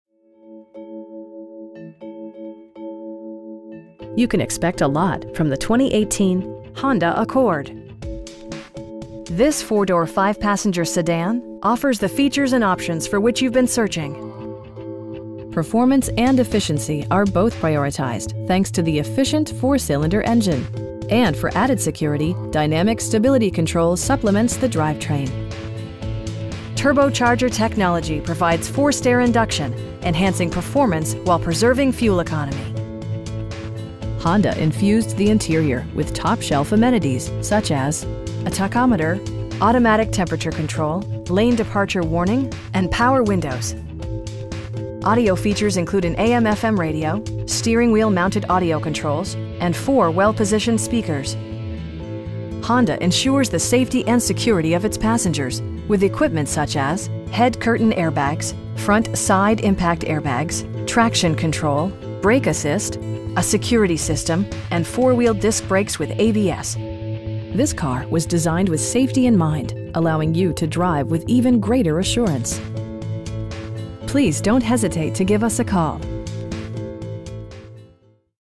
2018 Honda Accord LX in San Leandro, CA 94577
You can expect a lot from the 2018 Honda Accord. This 4 door, 5 passenger sedan offers the features and options for which you've been searching. Performance and efficiency are both prioritized thanks to the efficient 4 cylinder engine, and for added security, dynamic Stability Control supplements the drivetrain. Turbocharger technology provides forced air induction, enhancing performance while preserving fuel economy. Honda infused the interior with top shelf amenities, such as: a tachometer, front bucket seats, automatic temperature control, power door mirrors, lane departure warning, cruise control, and power windows. Audio features include an AM/FM radio, steering wheel mounted audio controls, and 4 speakers, providing excellent sound throughout the cabin. Honda ensures the safety and security of its passengers with equipment such as: head curtain airbags, front side impact airbags, traction control, brake assist, a security system, and 4 wheel disc brakes with ABS. This car was designed with safety in mind, allowing you to drive with even greater assurance. You will have a pleasant shopping experience that is fun, informative, and never high pressured. Please don't hesitate to give us a call.